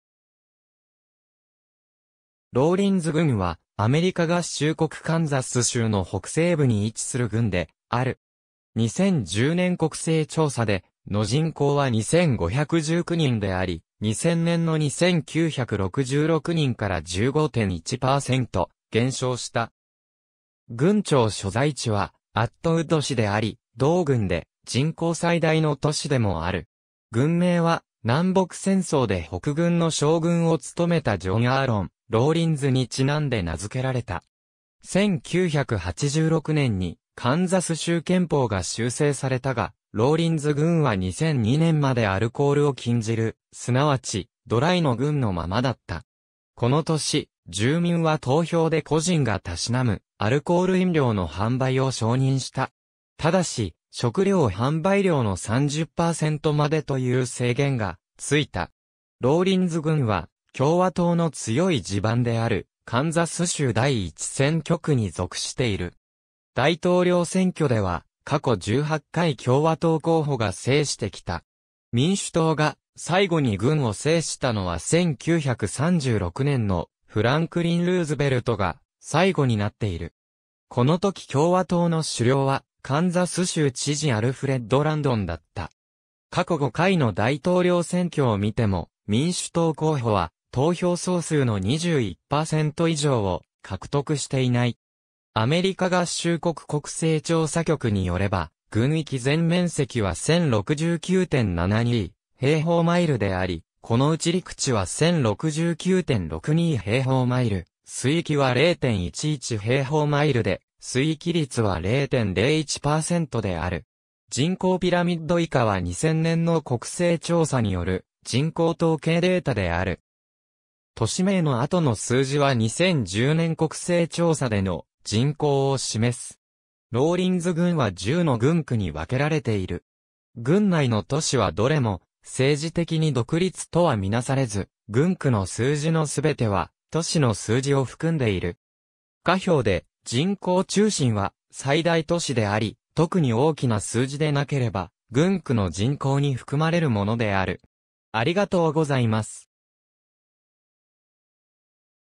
ローリンズ郡 (カンザス州)
ローリンズ郡（英: Rawlins County）は、アメリカ合衆国カンザス州の北西部に位置する郡である。
2010年国勢調査での人口は2,519人であり、2000年の2,966人から15.1%減少した。
郡庁所在地はアットウッド市（人口1,194人）であり、同郡で人口最大の都市でもある。
郡名は南北戦争で北軍の将軍を務めたジョン・アーロン・ローリンズに因んで名付けられた。
1986年にカンザス州憲法が修正されたが、ローリンズ郡は2002年までアルコールを禁じる、すなわち「ドライ」の郡のままだった。
この年、住民は投票で個人が嗜むアルコール飲料の販売を承認した。
ただし、食料販売量の30%までという制限が付いた。
ローリンズ郡は共和党の強い地盤であるカンザス州第1選挙区に属している。
大統領選挙では過去18回共和党候補が制してきた。
民主党が最後に郡を制したのは1936年のフランクリン・ルーズベルトが最後になっている。
このとき共和党の首領はカンザス州知事アルフレッド・ランドンだった。
過去5回の大統領選挙を見ても、民主党候補は投票総数の21%以上を獲得していない。
都市名の後の数字は2010年国勢調査での人口を示す。
ローリンズ郡は10の郡区に分けられている。
郡内の都市はどれも「政治的に独立」とは見なされず、郡区の数字の全ては都市の数字を含んでいる。
下表で「人口中心」は最大都市であり、特に大きな数字でなければ、郡区の人口に含まれるものである。